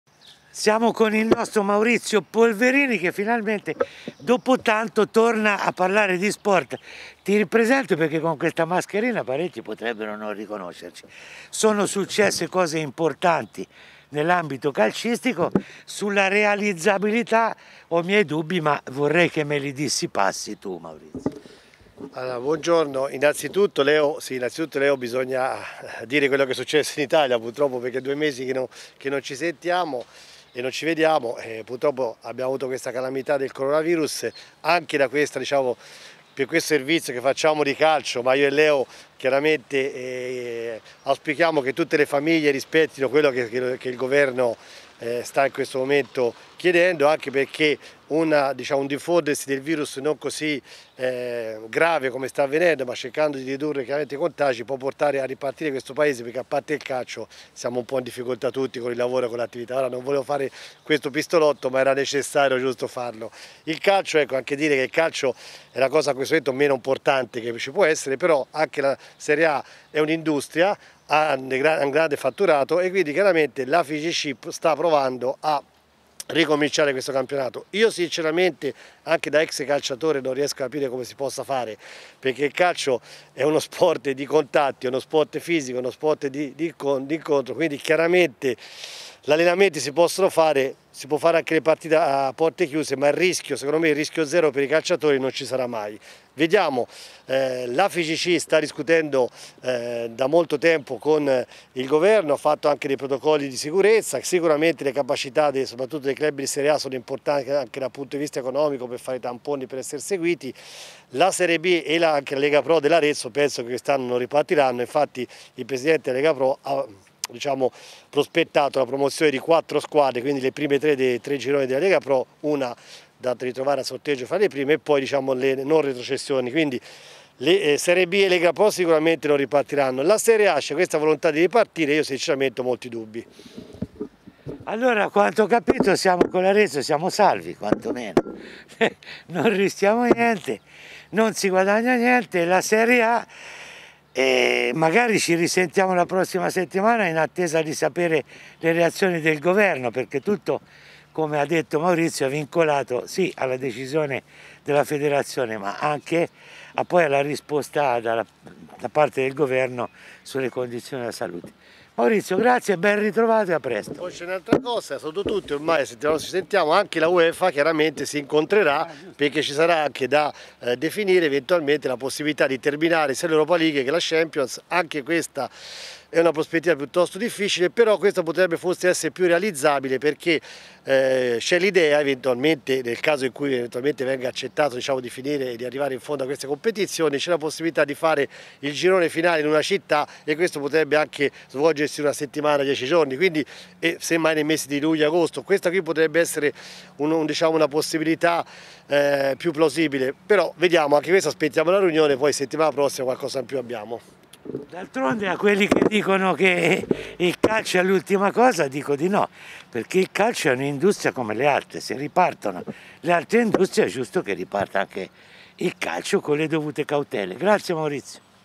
Coronavirus, il calcio al test del governo.
Da La Stampa:
Il governo  esprimerà il suo parere sulle procedure mediche di prevenzione predisposte dalla commissione della Figc. Il verdettoin una videoconferenza tra tutte le componenti calcistiche italiane – Federazione, Leghe, calciatori, allenatori e arbitri – con il ministro dello sport Spadafora. Difficile che arrivi il via libera necessario per riprendere ad allenarsi il 4 maggio. Più probabile uno slittamento di due settimane che allinei il calcio agli altri sport, in particolare agli allenamenti degli atleti individuali.
Spadafora, molto critico due sere fa sulla ripresa del calcio, non è convinto di alcuni punti del protocollo e avrebbe chiesto pareri esterni alla commissione. Non mancano le divisioni nemmeno interne al mondo del calcio. La Serie A, l’unica che potrebbe sostenere il protocollo avendo camere per i giocatori all’interno dei centri di allenamento oppure potendosi permettere di prenotare un hotel in esclusiva per settimane, ha appena perso il suo rappresentante medico, il dottor Tavana del Torino che si è dimesso. Sette club, tra cui Torino, Sampdoria e Brescia, hanno firmato un documento pieno di dubbi su alcuni aspetti del protocollo, in particolare sul rischio di un nuovo calciatore positivo. E ci sono dirigenti che ritengono nei fatti “inapplicabile” le misure anti-contagio del protocollo.
Lega B e Lega Pro non possono applicarlo per ragioni economiche e infrastrutturale, infatti la Figc ha deciso che in caso ripartiranno dopo la A. E i calciatori non vogliono passare per privilegiati, potendo accedere a tamponi e test sierologici (come previsto dal documento) al momento preclusi a gran parte della popolazione. Ieri l'Aic ha chiesto di evitare quest antipatiche differenze. Non è il viatico migliore per far scattare il semaforo verde del governo.
Ne parliamo con Maurizio Polverini a cui diamo il nostro bentornato!